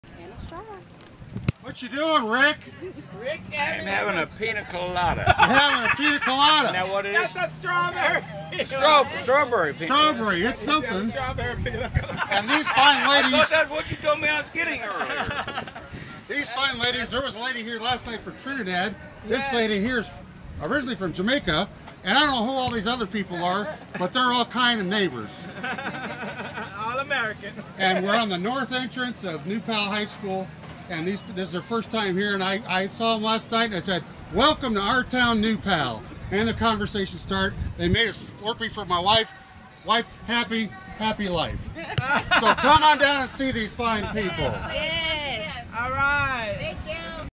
NewPalSummerFest Tropical Smoothies by Chris’s Kitchen Gadgets
Tropical Smoothies by Chris’s Kitchen & Gadgets - a booth called “Tropical Smoothies” was at the New Pal Summer Fest in New Palestine, Indiana. My wife noticed customers walking away from their booth with brightly colored beverage glasses.

Welcome to OurTownNewPal !

It turns out they’re heavily involved in the kitchen gadget arena!

Video